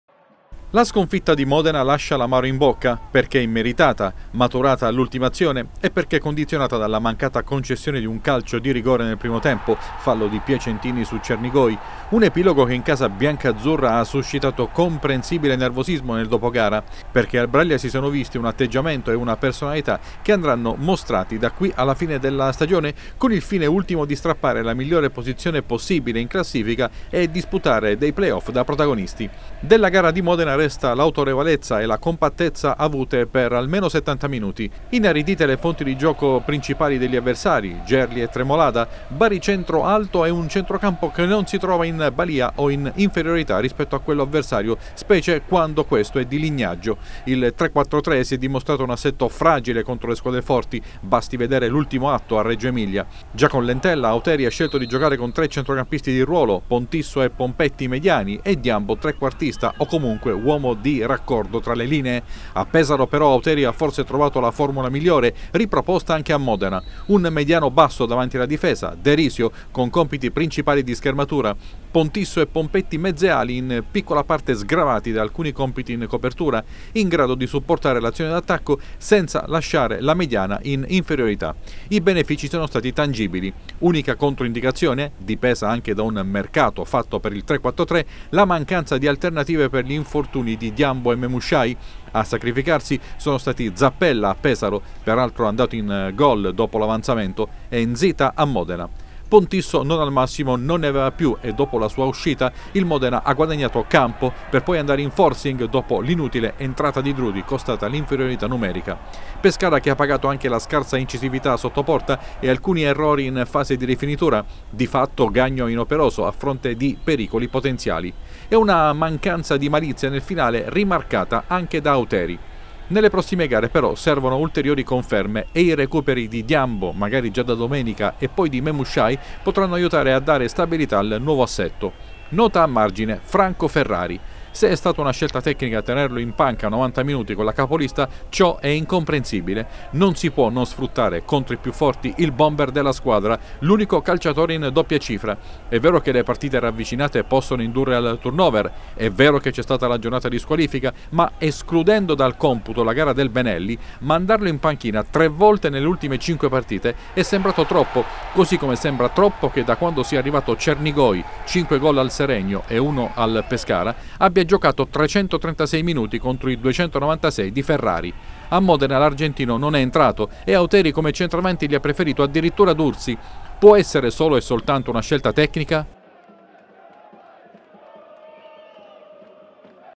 Pescara autorevole: tracciata la strada per il finale di stagione
Alessandro Sorrentino ha sottoscritto stamani il suo primo contratto da professionista col Pescara. Il giovane portiere ha firmato per 5 anni. Intanto resta ancora l’amaro in bocca in casa Pescara per il ko in extremis a Modena. Ma i progressi visti nelle ultime due partite sembrano la base su cui il delfino può imperniare la parte finale del campionato, a partire dalla gara di domenica all’Adriatico contro il Siena